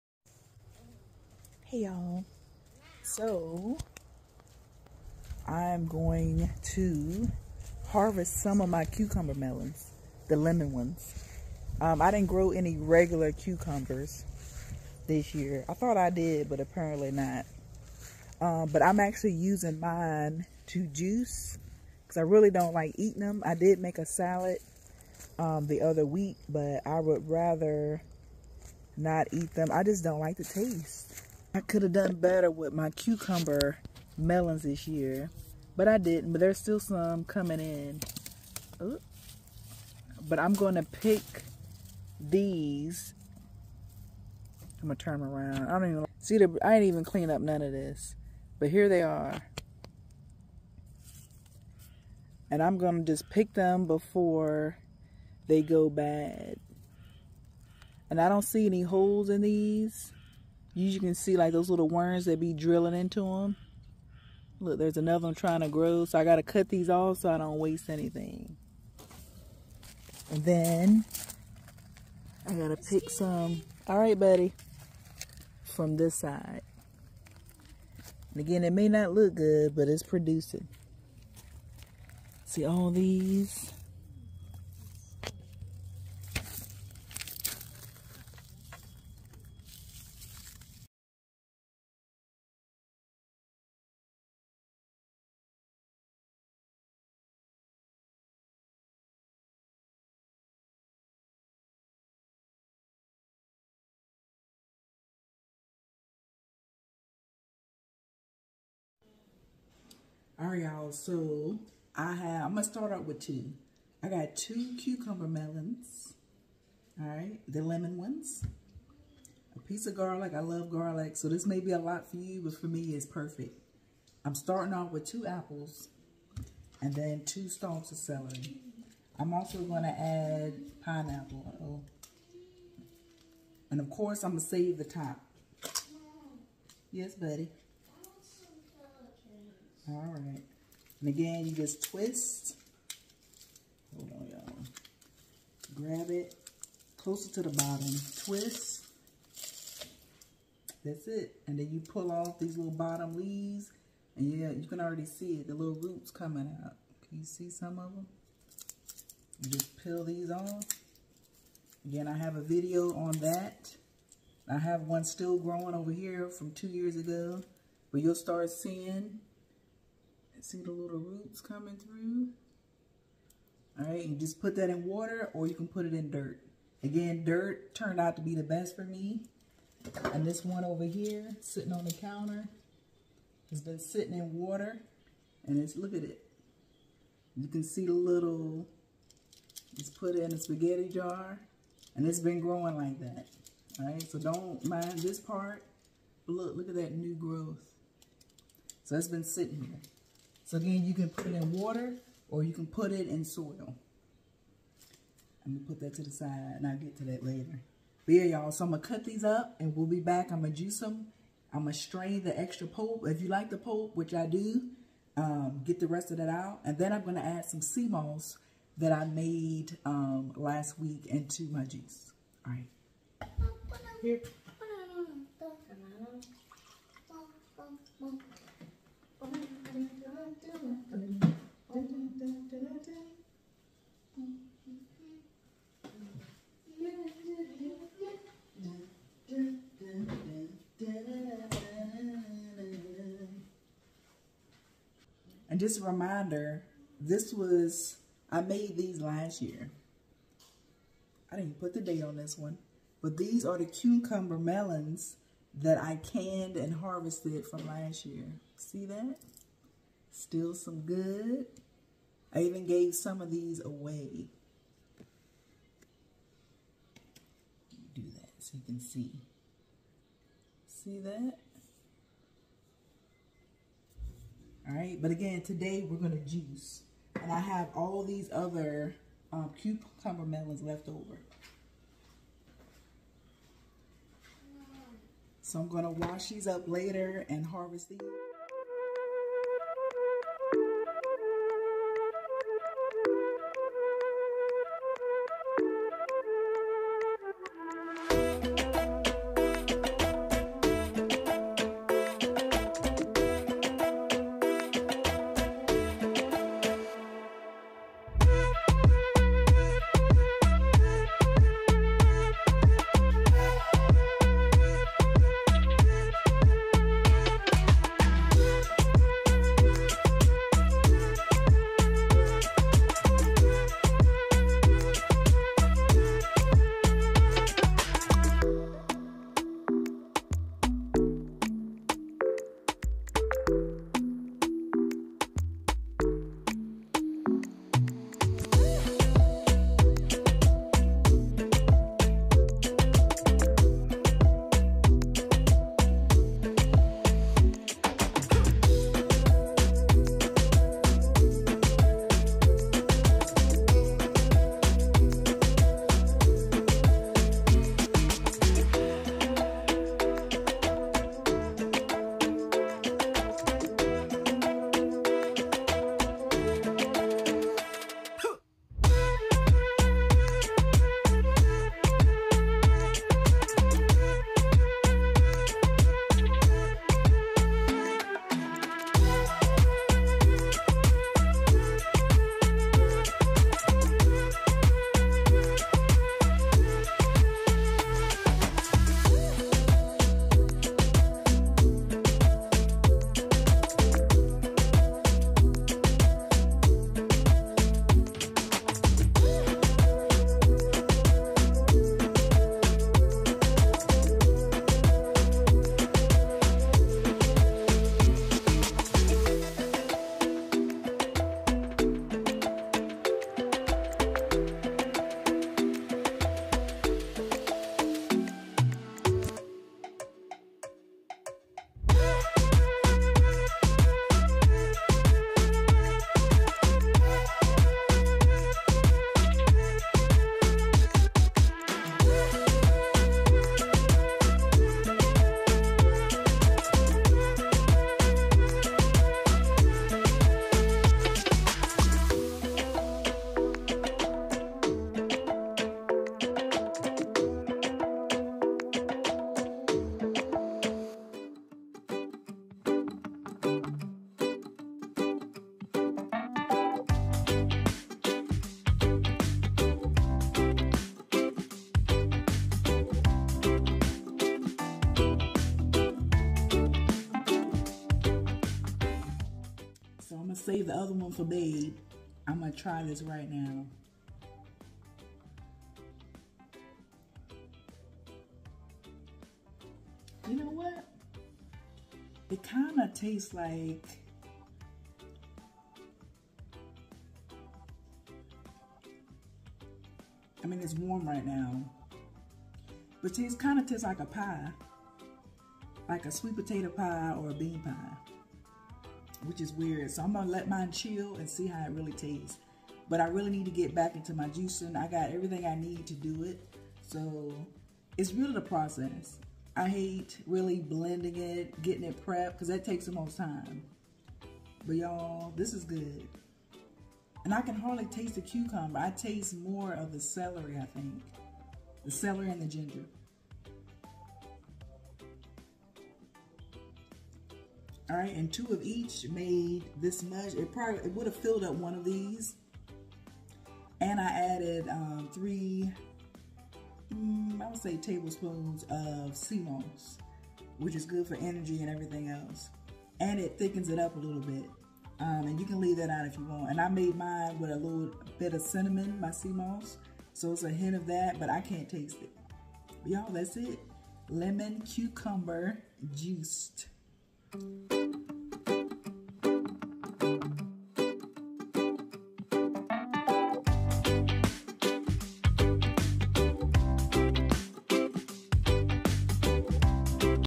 Juicing Lemon Cucumber Melons
These are Lemon Cucumber Melons & quite easy to grow! They Trelis and produce a lot of fruit! Each one has hundreds of seeds, so you never have to buy any more!

Save the pineapple scraps, yes even the skin. Grab some more ginger & turmeric then boil away!⬇️
Traditional Jamaican Pineapple Ginger & Turmeric Drink